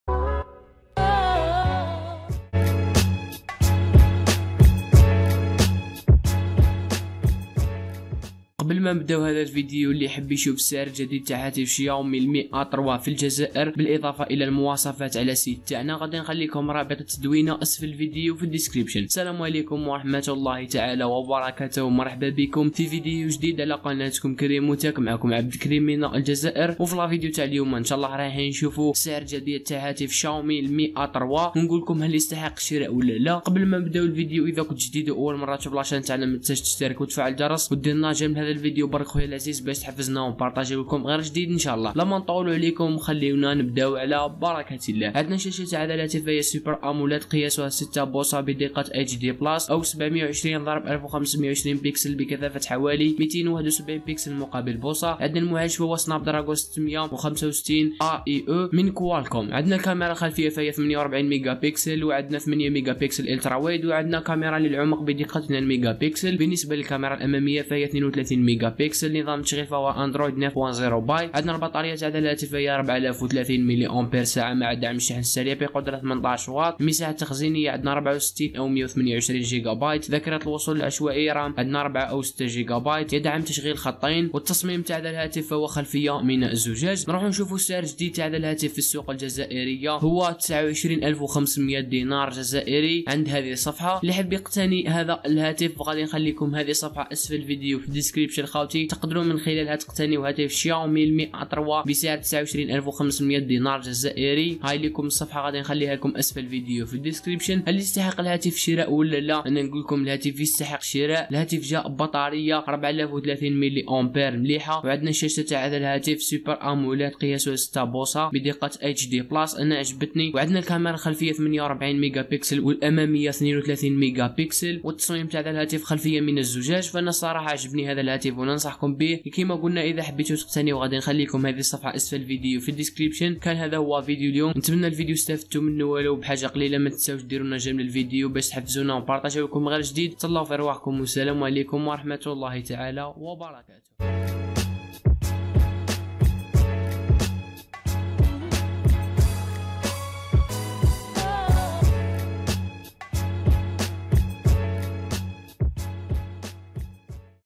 تخفيض جديد على سعر هاتف Xiaomi Mi A3 في سوق الجزائر l هل يستحق الشراء أم لا ؟؟
مرحبا بكم خاوتي ^_^
● موضوع هذا الفيديو :
في هذا الفيديو الجديد إن شاء الله رايحين نشوفو السعر الجديد نتاع هاتف شاومي Mi A3 بعد ما طاح السعر نتاعو في سوق الجزائر و نعطيكم الرأي نتاعي فيه أي هل يستحق الشراء أم لا ؟؟ .

السعر الجديد في الجزائر : 29500 دج .

🌟 متنساش تشترك بالقناة و تفعل الجرس باش يوصلك كل فيديو جديد + دير جام لهذا الفيديو و شكرا بزاف 😎
● قناة كريمو تيك " آخر أخبار التكنولوجيا و أسعار الهواتف الذكية في الجزائر